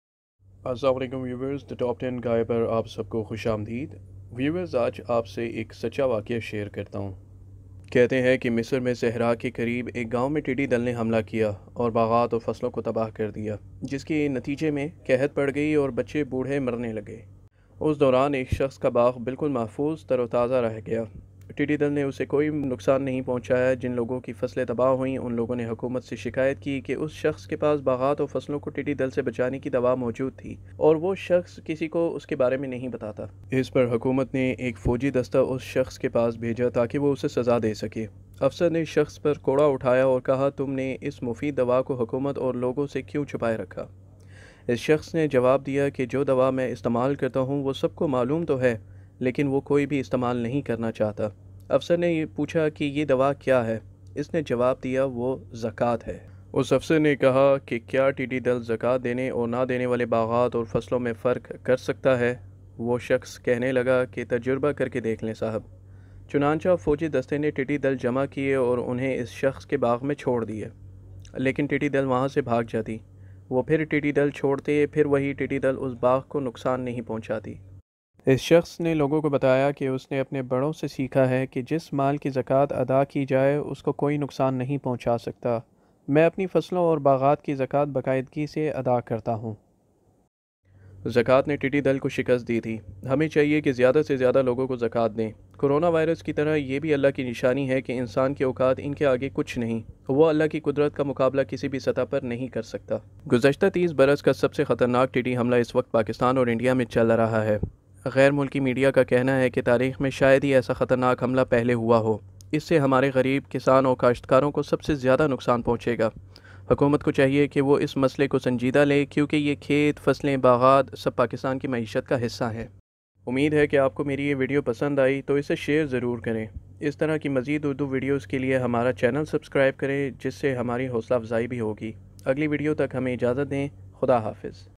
LOCUST ATTACK, IMPORTANCE OF ZAKAAT / ٹڈی دل کا حملہ اور زکات کی اہمیت
Today I have shared a true event. Being a Muslim, practice Zakat as much as you can. We pray for Pakistan that may Allah protect it from any catastrophy or wars. Ameen

English subtitle will be uploaded shortly.